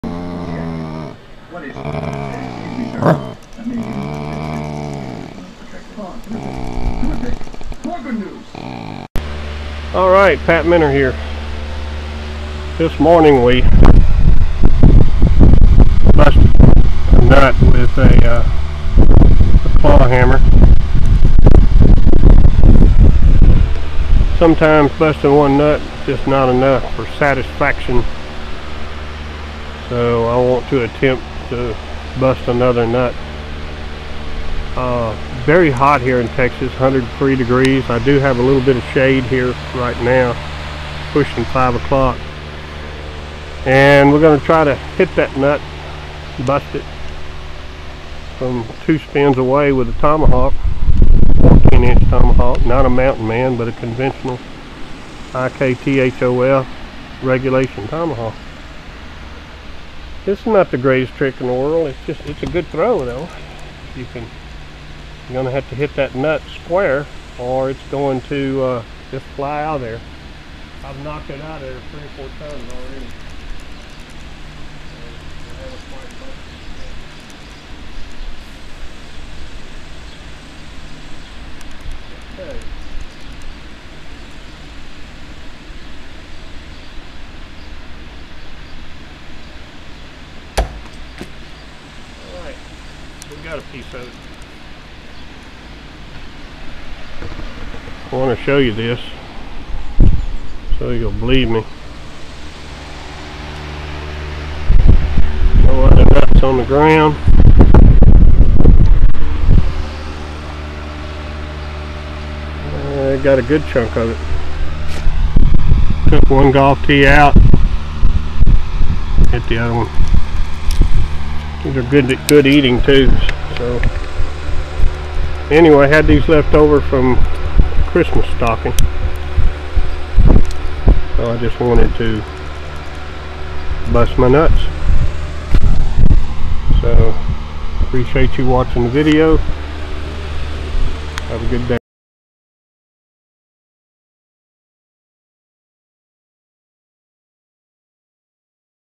Busting My Nuts
Busted a nut this morning with a claw hammer from 13 feet away,so I decided to bust another nut with a tomahawk from over 20 feet away